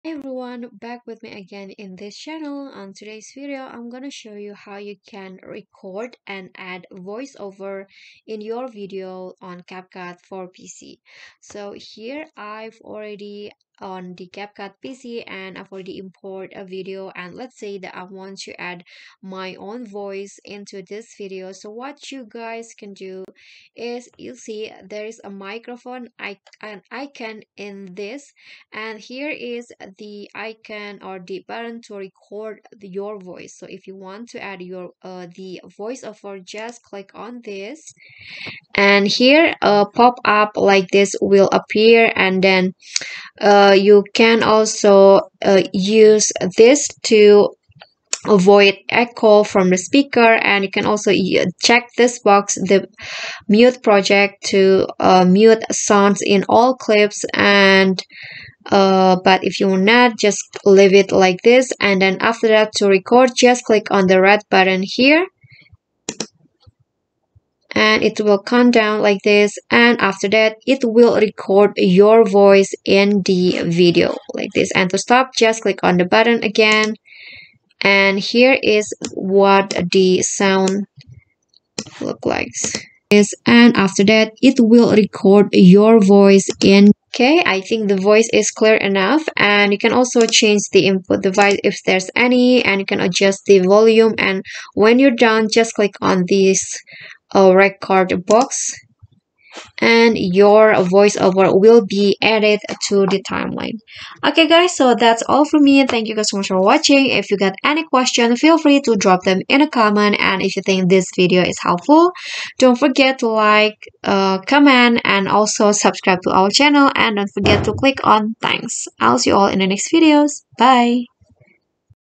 How to Record & Add Voice-Over on CapCut PC
Here is how you can record and add voice-over for your video in Capcut for PC